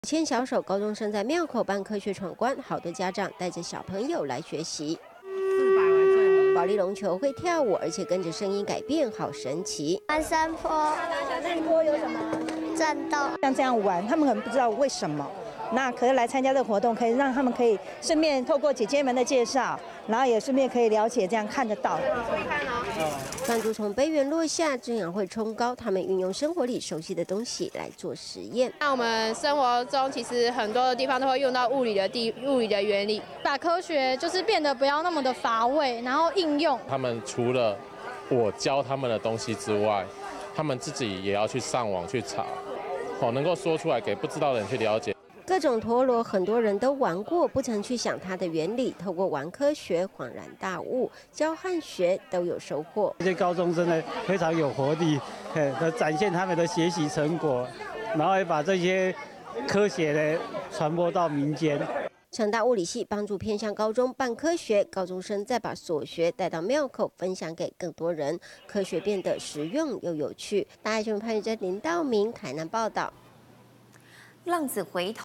學生承先啟後 廟口玩科學推教育
推廣偏鄉科學教育，台南市成功大學師生到偏鄉的國中舉辦科學研習，訓練種子小老師，再到廟口辦科學活動，將科學理念融入到生活裡面，讓親子輕輕鬆鬆認識科學。